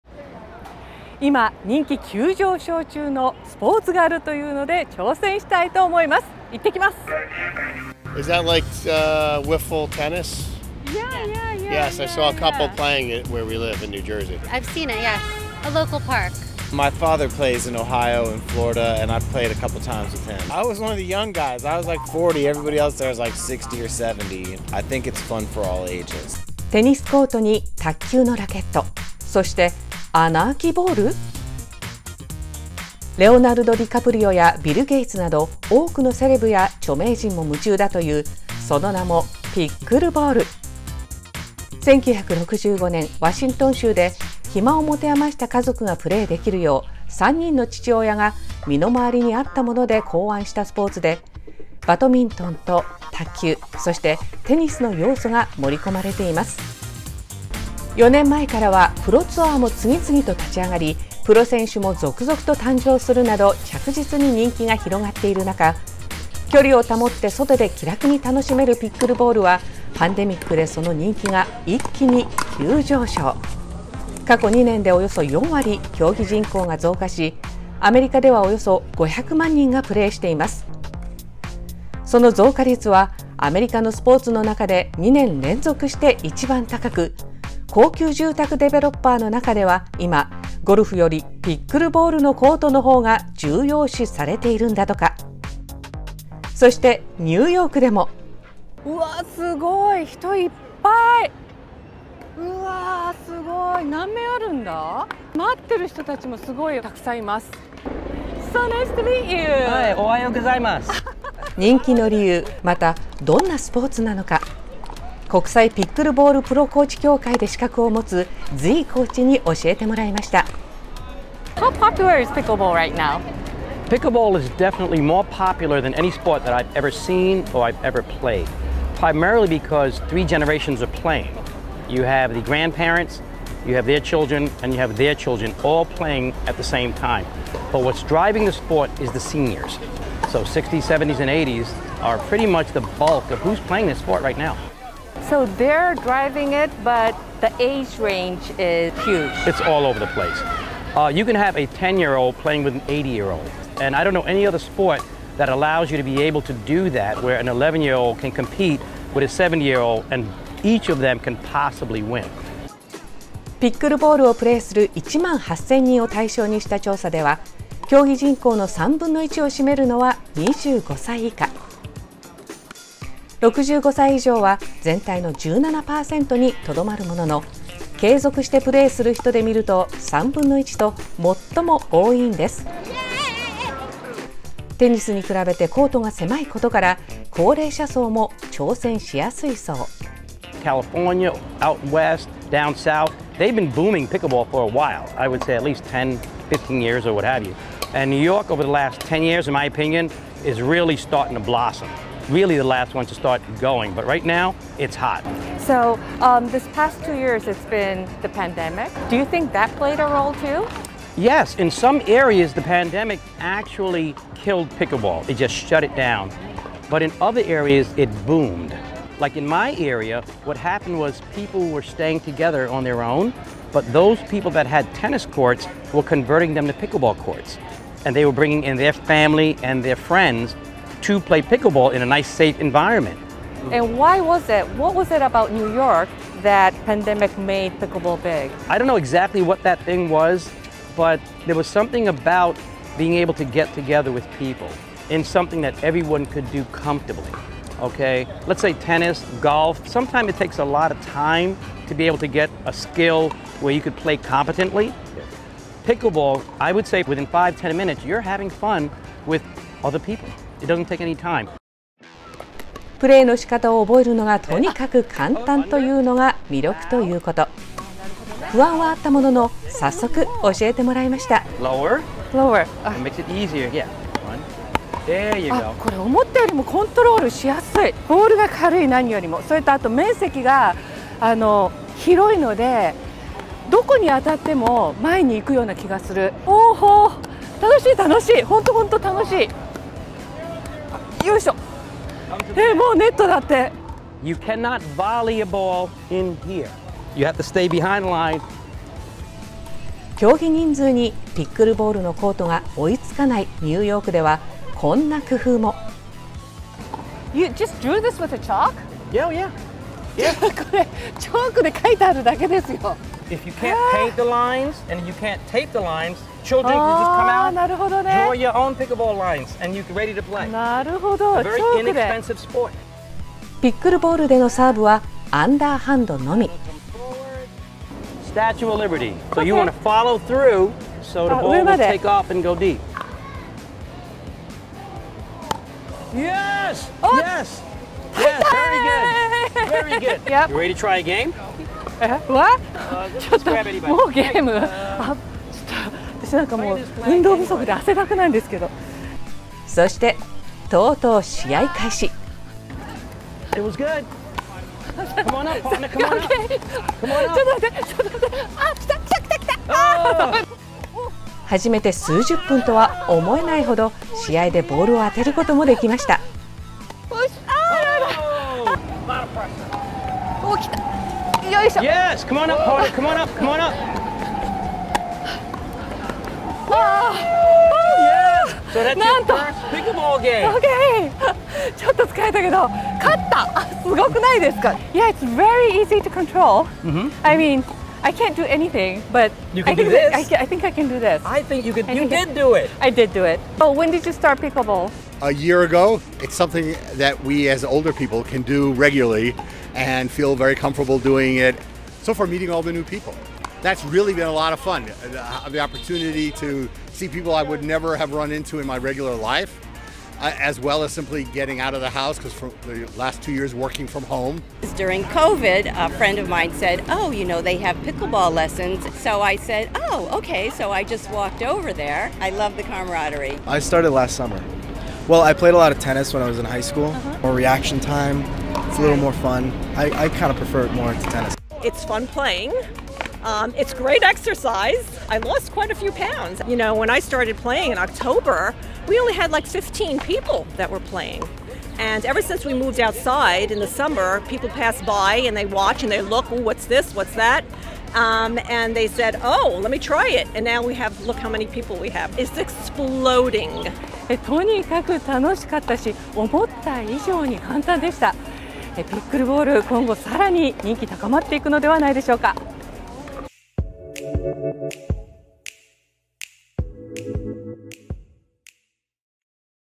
ニューヨークでも人気急上昇！アメリカ発祥のスポーツ「ピックルボール」
バドミントンと卓球とテニスの要素を合わせたスポーツ、「ピックルボール」（Pickleball）。今や世界各国でもプレーされるようになった人気急上昇中のこのスポーツの人気の理由をIPTPAで資格を持つ"Z"コーチに聞きました。そして、久下キャスターも初めてのピックルボールを全力で体験します！